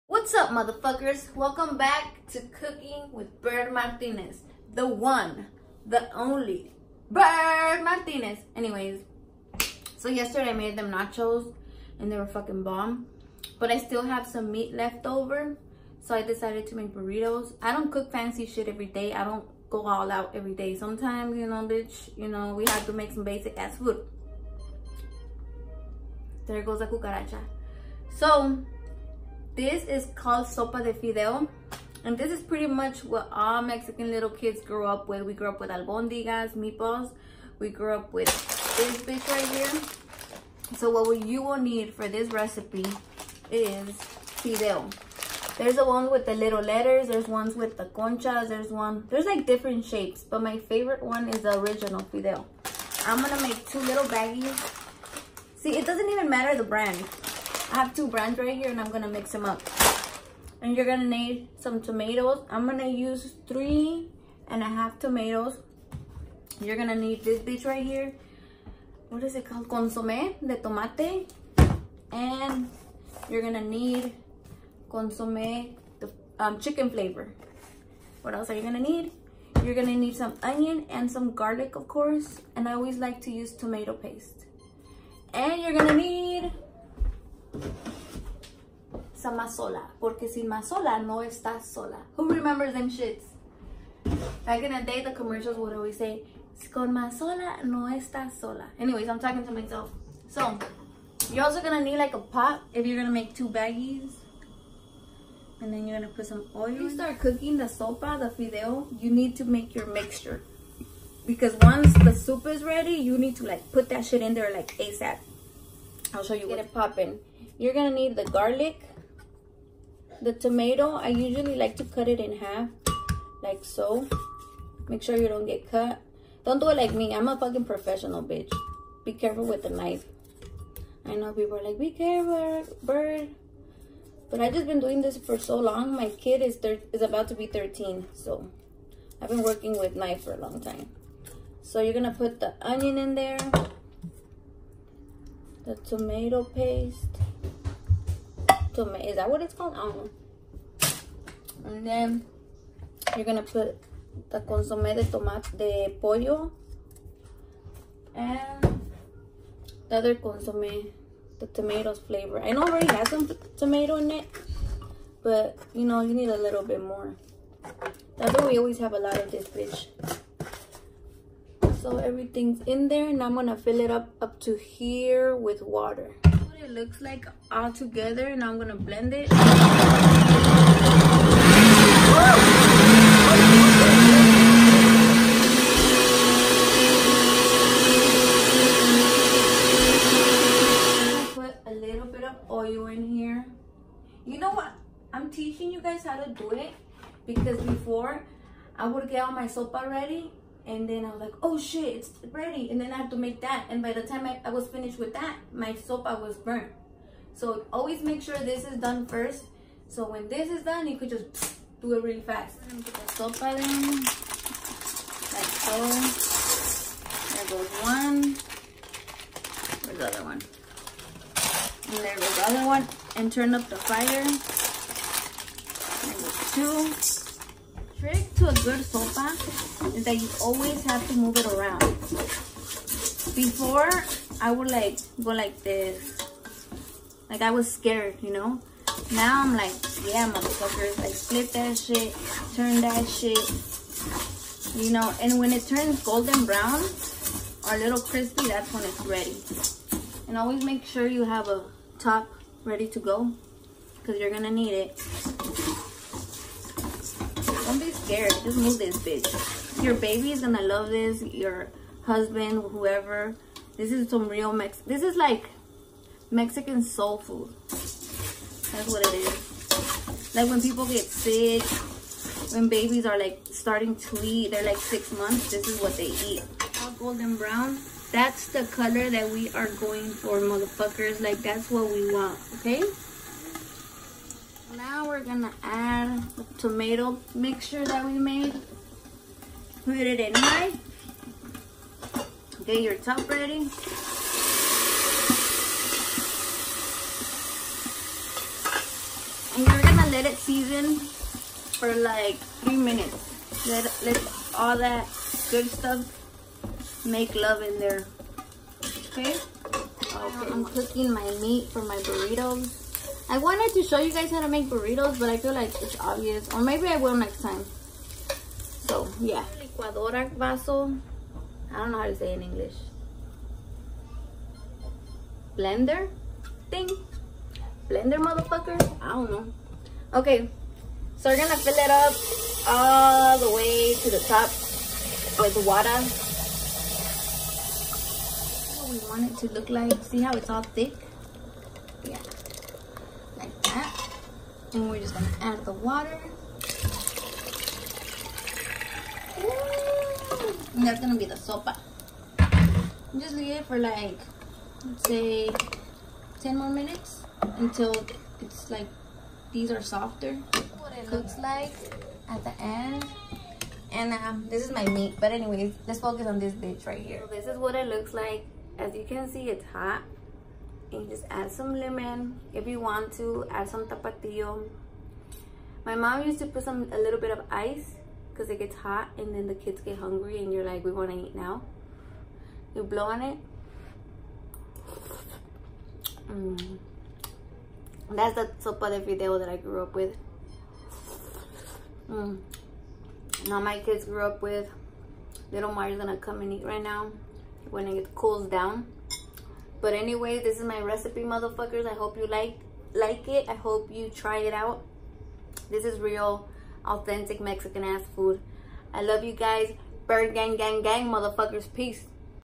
Recipe for Mexican Fideo soup (sopa de fideo)
For this recipe I used,
2 bags of cut fideo (la morena brand)
4 tomatoes
Onion
Garlic cloves
Chicken and tomato flavor bouillon (Knorr brand)
Tomato paste

Peace and Mfkn love!